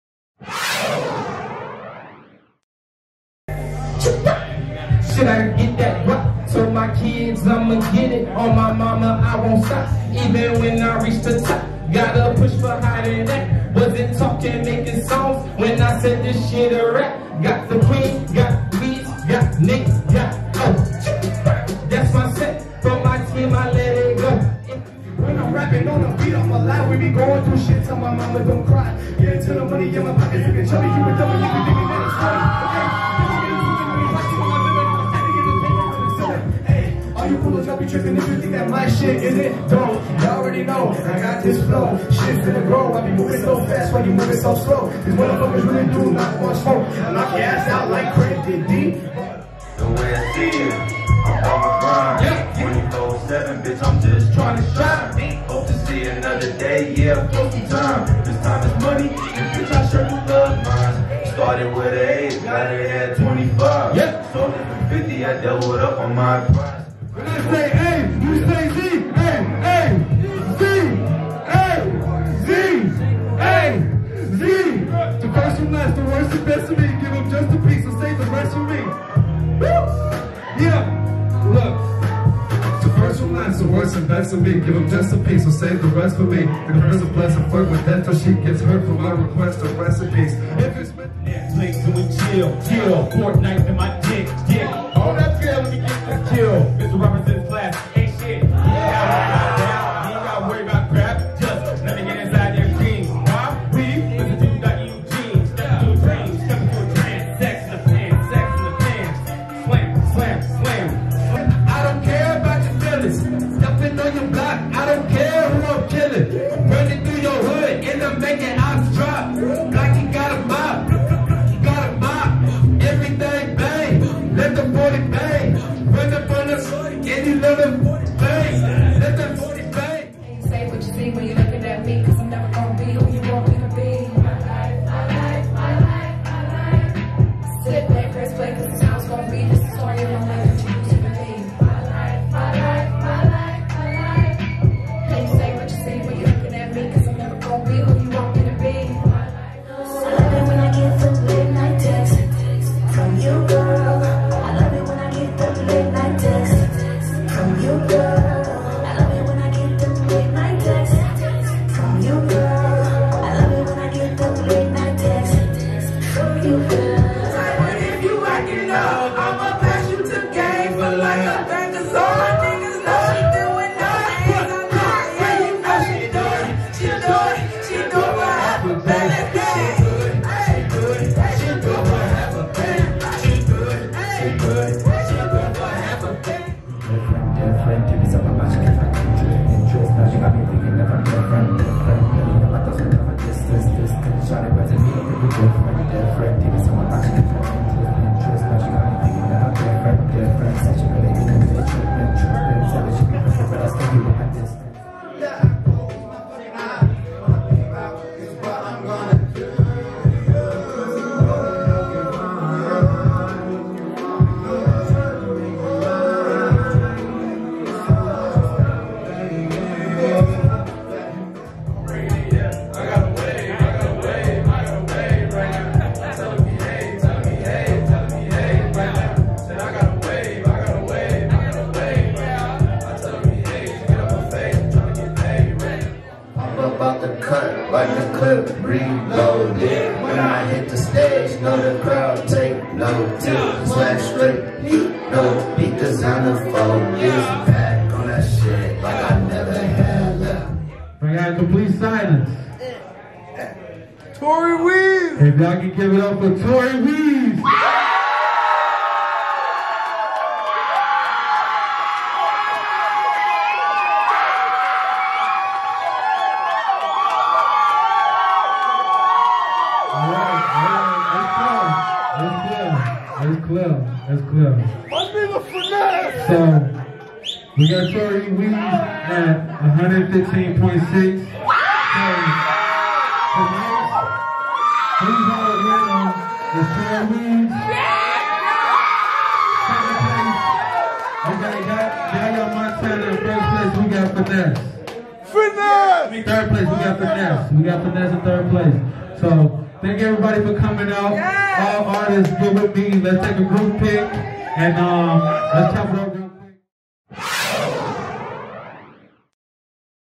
RECAP - FEB. 8TH | "WHO'S HOTTA" Open Mic Competition
Shot by @ArKanaEnt

Artist List:

Swiss Sweets
Yung J
Oh
Shane Cory
Doctor Eugene
Yaya Montana
Tori Weeds
B.R.E.E
Hesus Da God & TDT Music Group
Undead Exspijravit
Tony King
Fynesse
Quote The Don